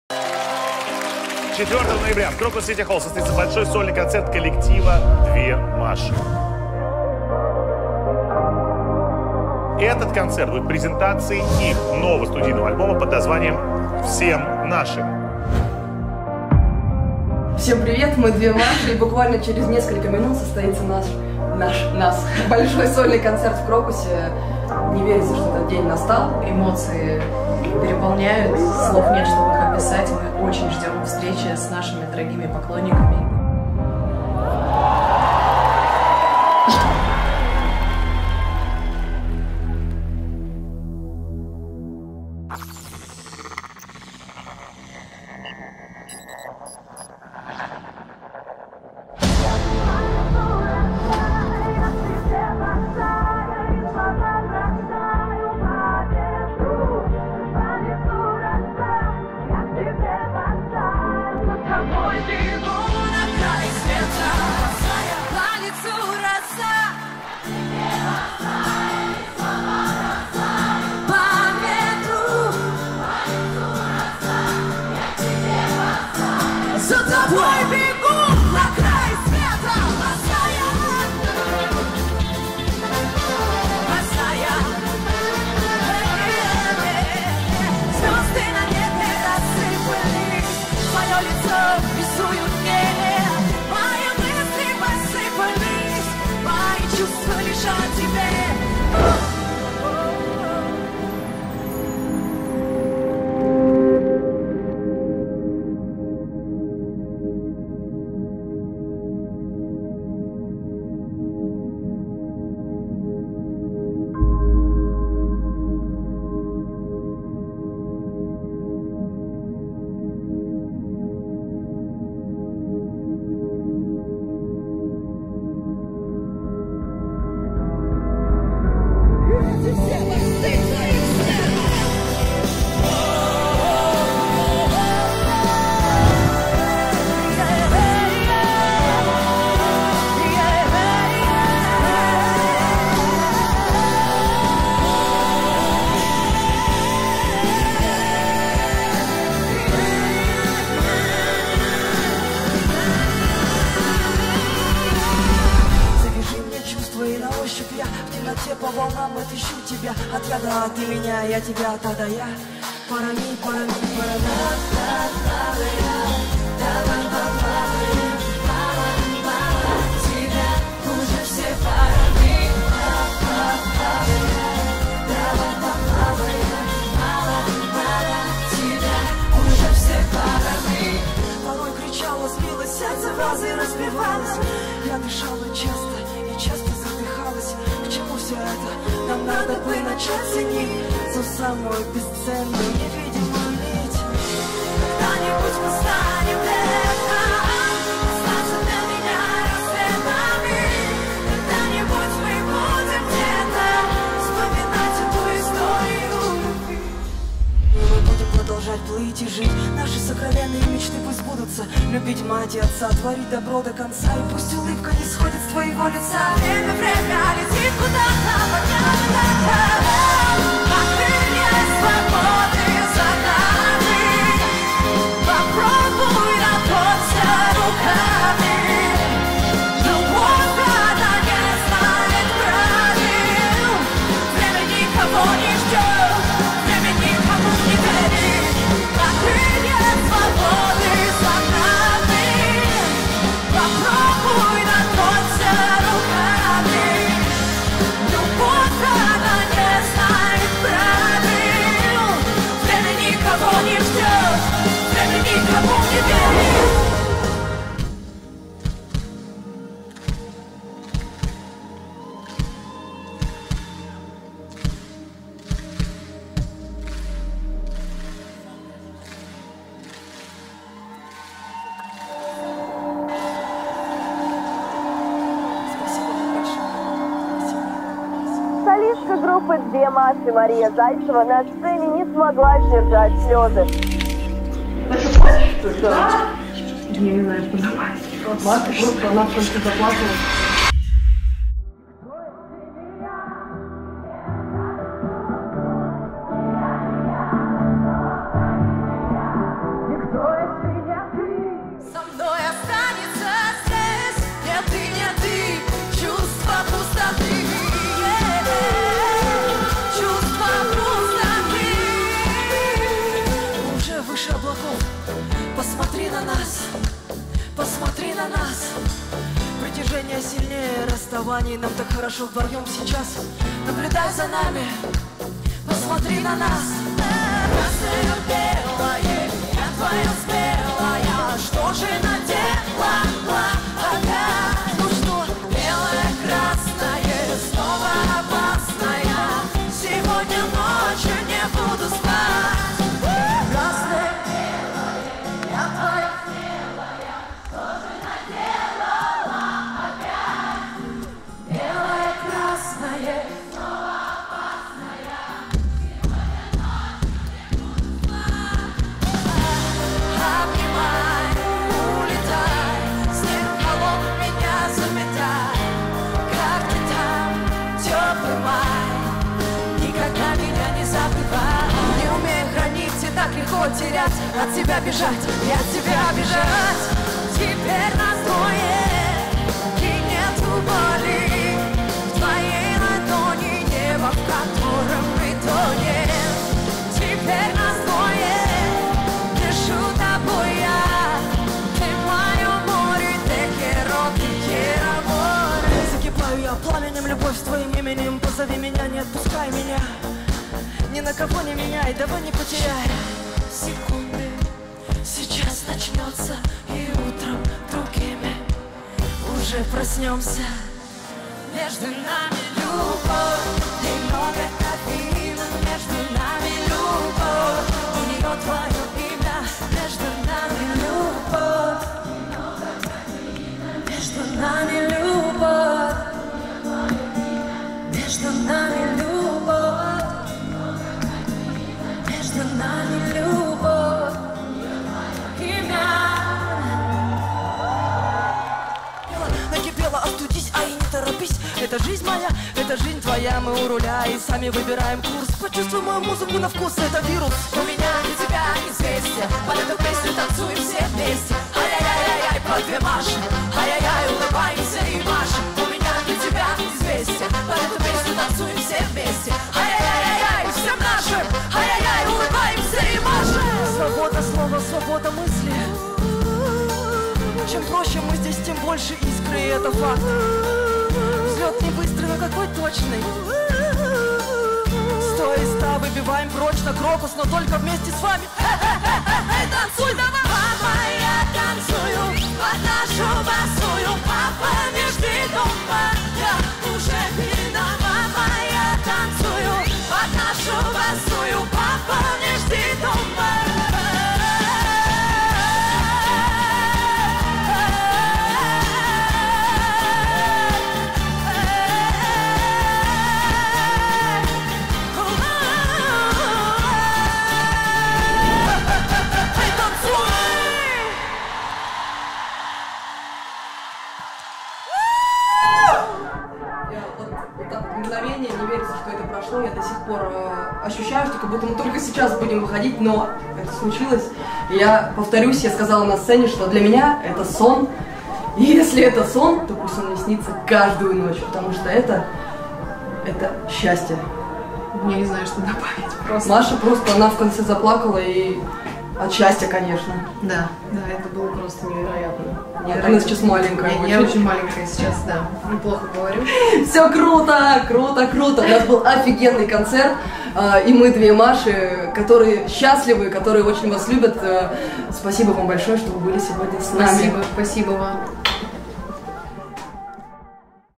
2Маши “Крокус Сити Холл 2019“ репортаж
С радостью делимся с вами этим видео. Это фрагменты с нашего большого сольного концерта в Крокус Сити Холл 4 ноября. До сих пор переполняют эмоции от того, что произошло между нами с вами в тот день... Sold out в Крокусе - мы об этом мечтали ! И да. И спасибо, что вы у нас есть! Ваши